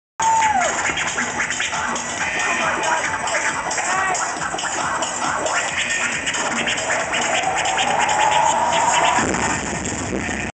Dj Blade at Phoenix "My Friend's Jason's other house"
small snippet of me scratchin to some speed garage